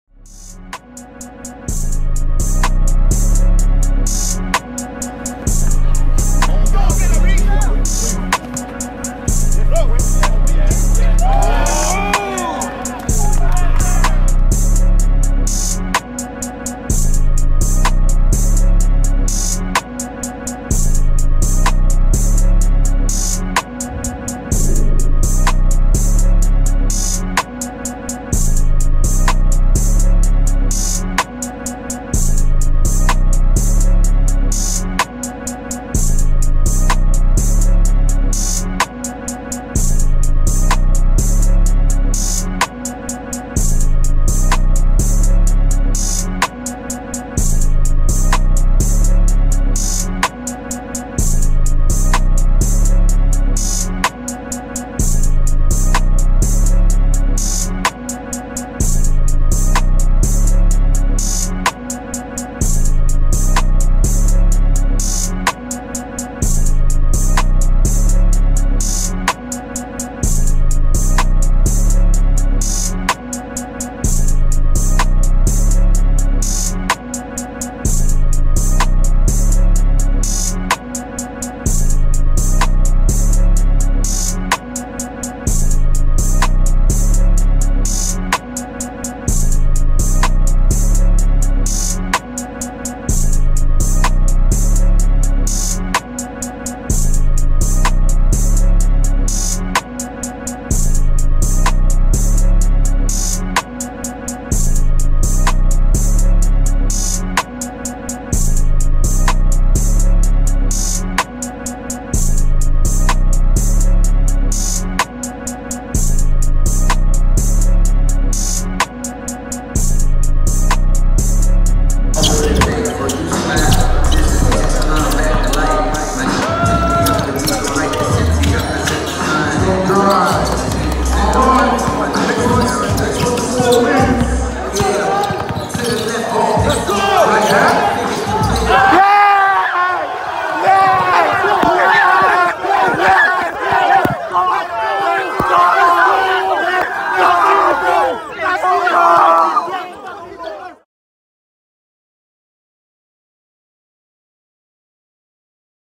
Mo Adepoju Ball Out Highlights 2021
National Championships took place on Sunday 15th of August 2021.
Mo Adepoju Dynamic Combo Guard.
98 Born.